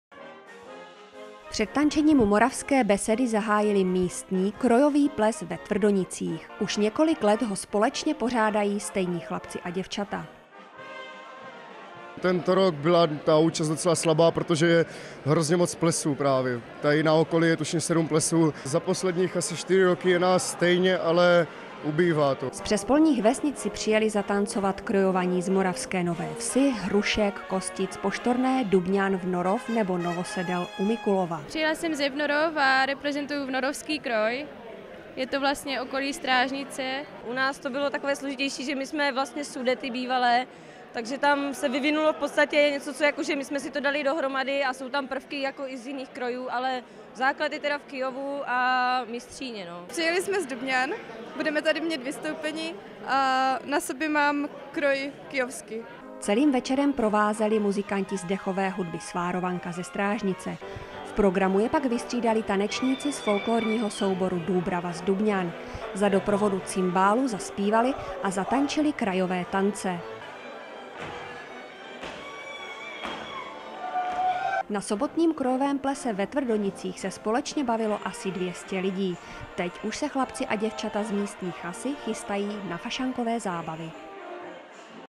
Krojový ples Tvrdonice RTVJ
Tvrdonice/ Moravská beseda, krásné kroje ani dobrá nálada nemohly chybět na krojovém plese ve Tvrdonicích. Ples si nenechali ujít ani krojované páry z Moravské Nové Vsi, Hrušek, Kostic, Poštorné, Dubňan, Vnorov či Novosedel na Mikulovsku.

„Přijeli jsme z Dubňan a budeme tu mít dnes večer vystoupení,“ těšila se Markéta Skočíková v bohatém Kyjovském kroji.

K tanci i poslechu hráli návštěvníkům muzikanti z dechové hudby Svárovanka ze Strážnice a o zábavu se postaral folklorní soubor Dúbrava z Dubňan. Za doprovodu cimbálu představil krajové tance.

Přestože se ve Trvdonicích bavily asi dvě stovky plesových hostů, tamní chasa čekala návštěvníků více. „Letos je účast docela slabá, protože v okolí je nabídka plesů hodně široká,“ poznamenal tvrdonický stárek Vít Šlichta. (nej)